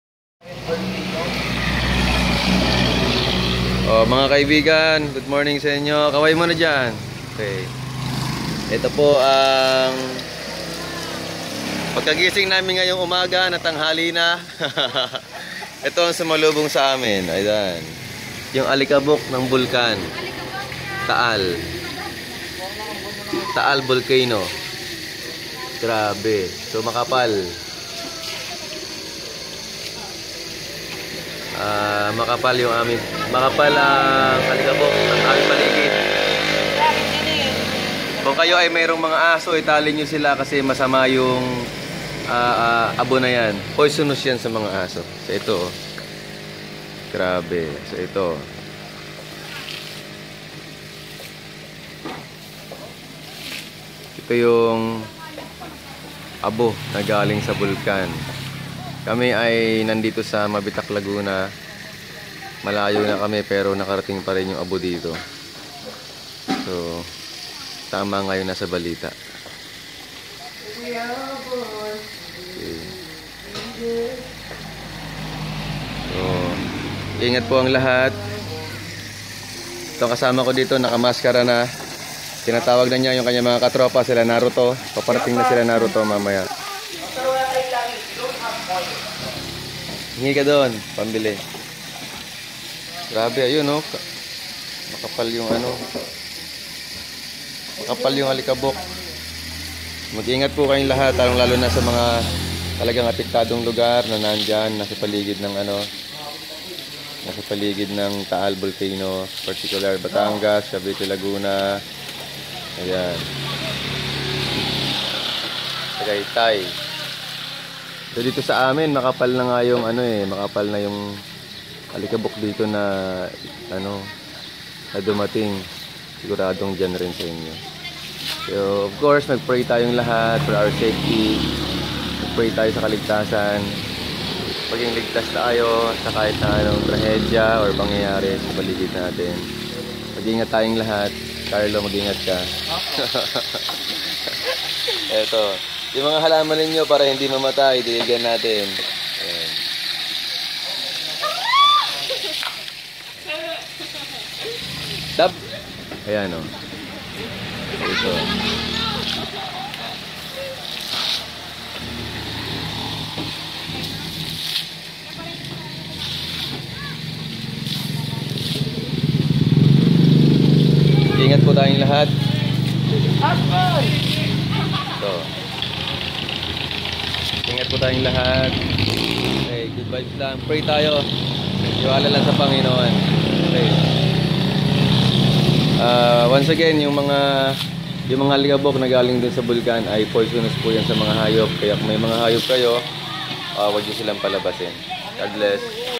Taal Volcano Ashfall | eruption... Dangerous to your Dog!
Eruption of Taal volcano and it's ashfall!